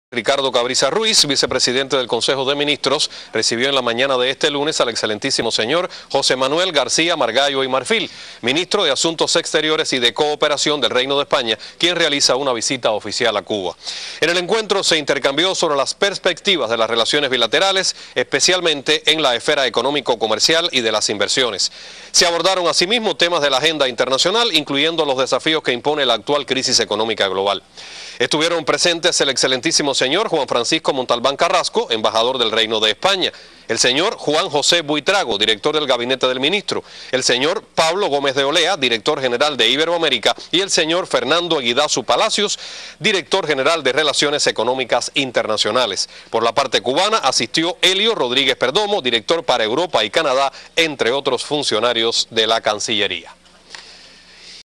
Llega a Cuba Canciller de España
El ministro español de Asuntos Exteriores, José Manuel García-Margallo, llega a La Habana en su primera visita oficial a Cuba, con el objetivo de pasar revista a la relación bilateral y a temas internacionales como la próxima Cumbre Iberoamericana o la negociación entre la isla y la UE.
El Canciller español sostiene conversaciones con el vicepresidente del Consejo de Ministros de Cuba,  Ricardo Cabrisas Ruiz.